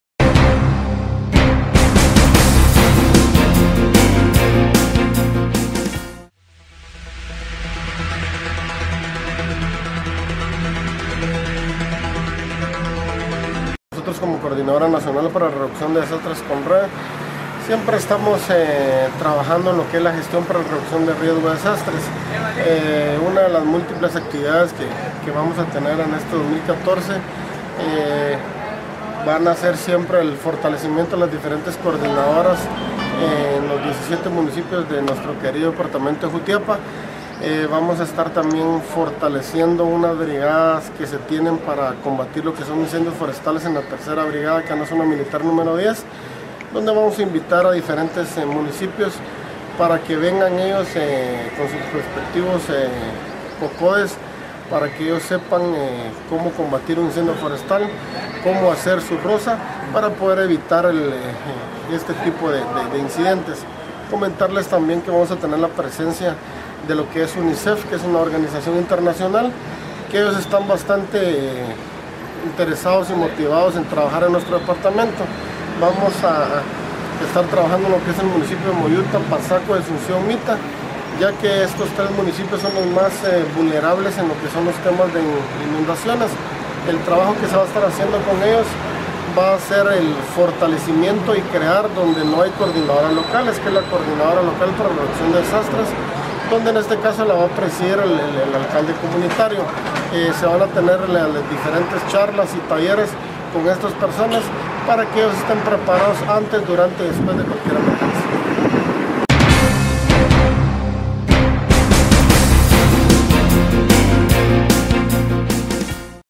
CONRED Propone Actividades para la Prevención de Desastres
El delegado de CONRED en Jutiapa nos habla sobre varias actividades que realizarán durante el año 2014 en la que se encuentran varias capacitaciones para la prevención de desastres.